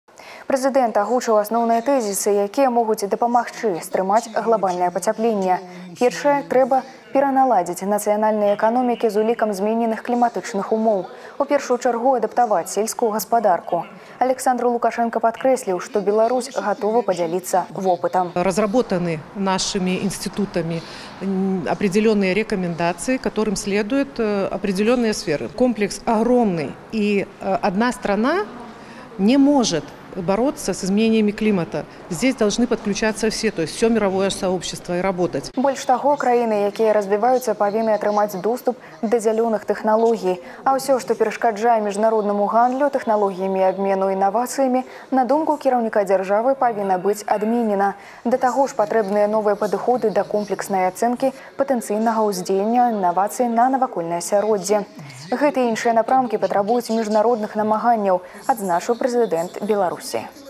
Лукашенко на Всемирном саммите в Баку озвучил тезисы, которые помогут сдержать глобальное потепление
Первое – нужно перенастроить национальные экономики с учетом измененных климатических условий.

В первую очередь адаптировать сельское хозяйство. Александр Лукашенко подчеркнул, что Беларусь готова поделиться опытом.